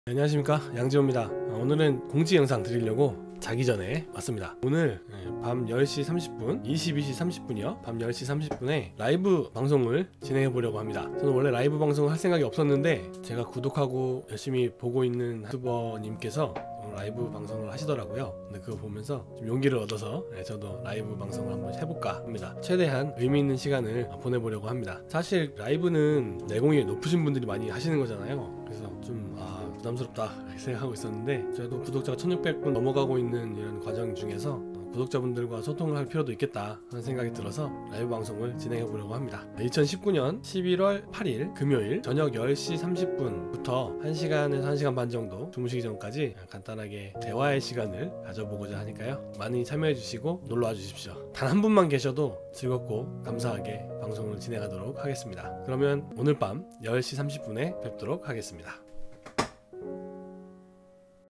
양재호TV 첫 라이브 방송! 2019.11.08(금) 저녁 10시 30분부터!
어떤 주제도 좋습니다.
단 한분만 계셔도 즐겁게 이야기 나누겠습니다.
편하게 대화하는 시간이 되었으면 합니다~^^